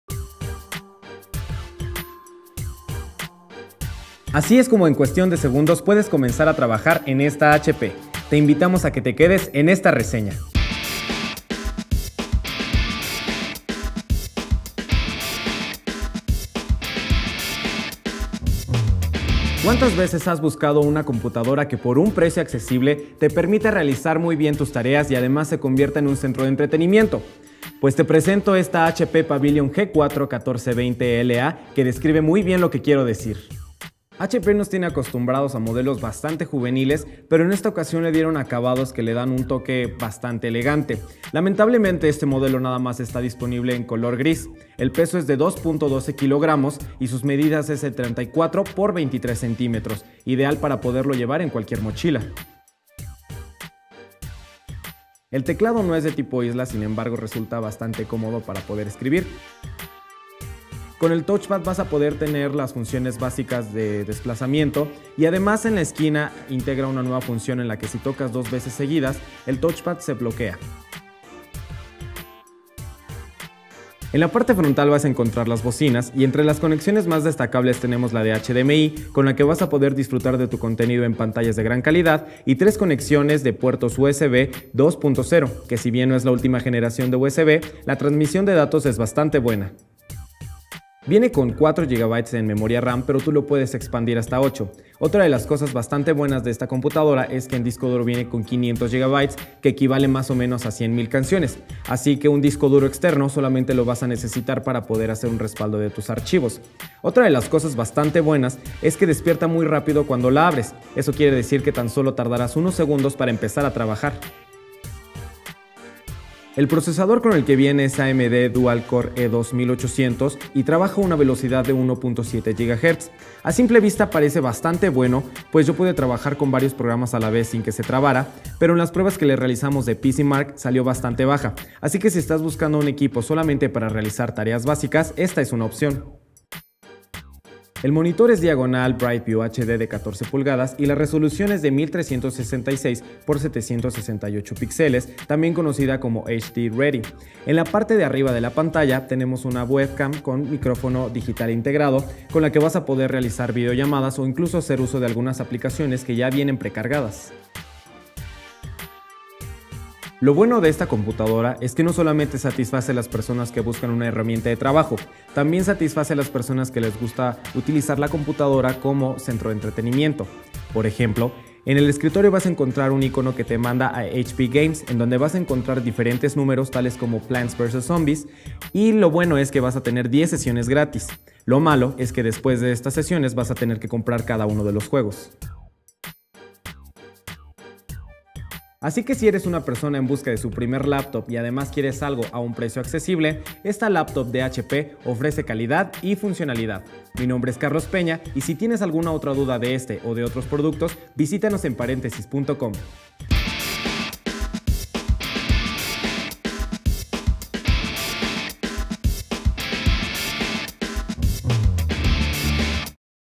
Laptop HP Pavilion (G4-1420la)
Por sus características: diseño, peso, capacidades y precio, esta notebook es ideal para estudiantes o personas que están buscando comprar su primera computadora portátil.